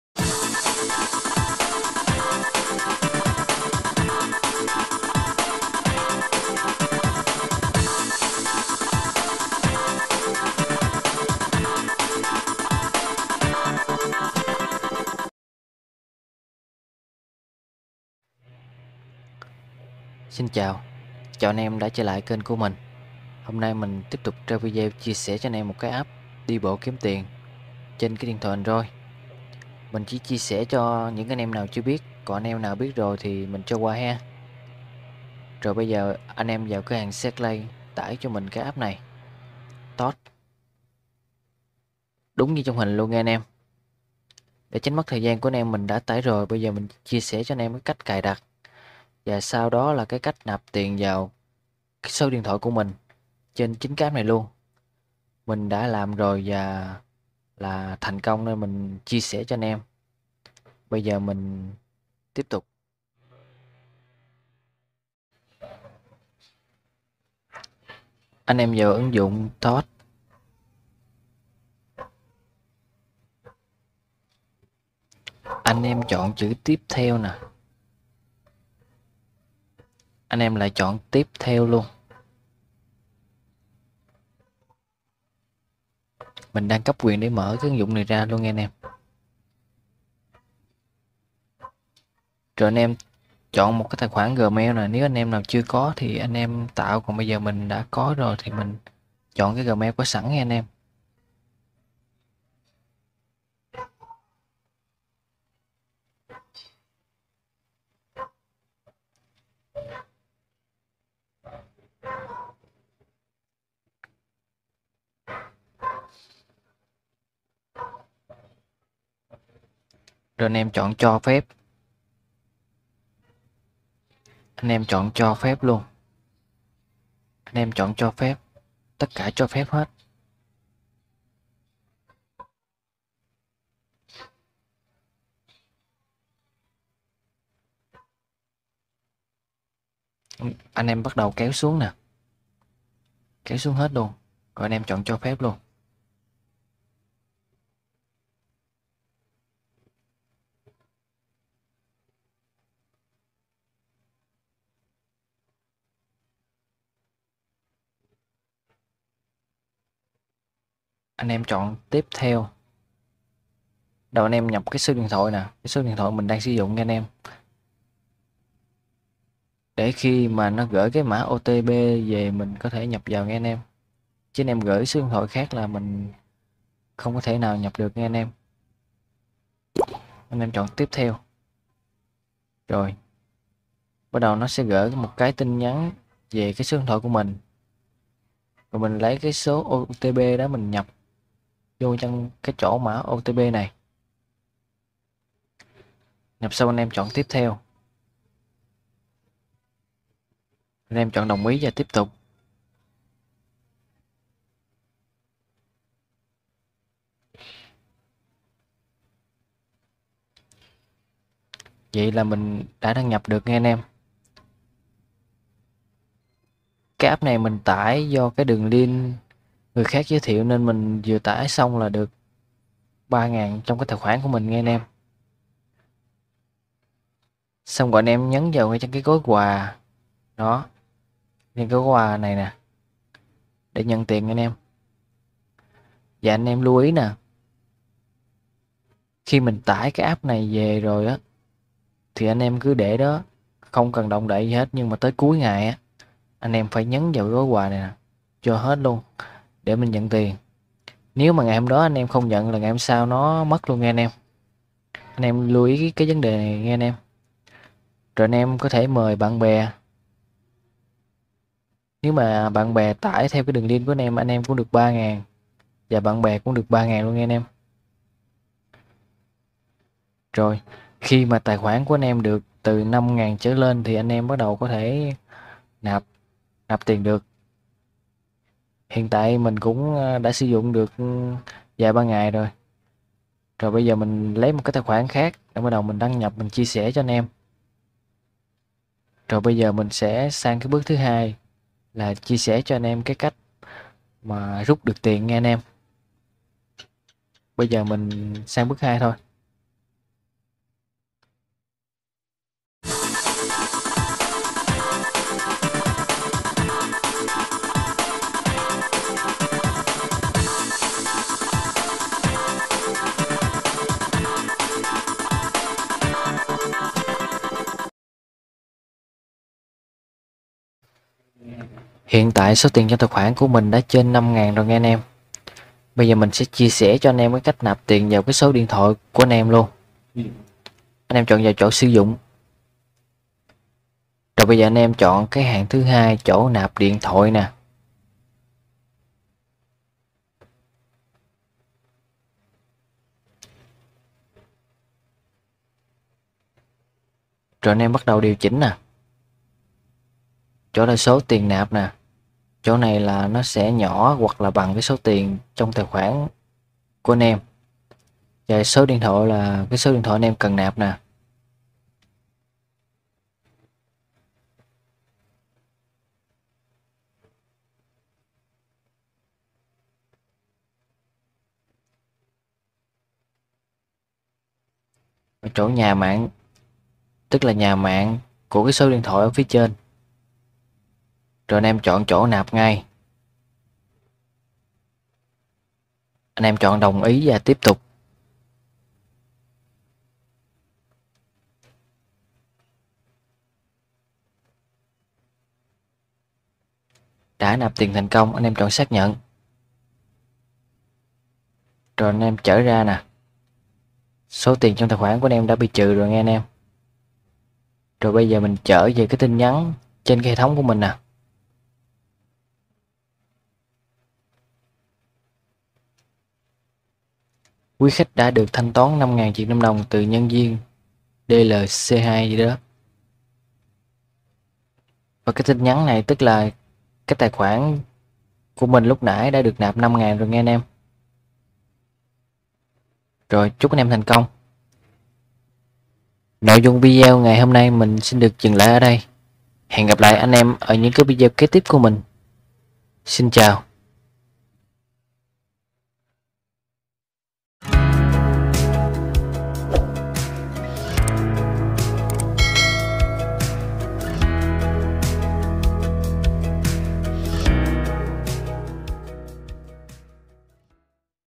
Đi bộ kiếm tiền là có thật
Phạm Tâm vlogss chia sẻ cho anh em app đi bộ kiếm tiền trên điện thoại Android một trải nghiệm tuyệt vời, thật sự mình đã nạp dc tiền từ app này, nội dung video không phải quảng cáo cho bất cứ thương hiệu nào cả chúc anh em thành công!!!
Toss - Kiếm tiền mỗi ngày
Bạn chưa cài Toss ư? - Ấn vào link và cài Toss để nhận ngay 7.000đ nhé!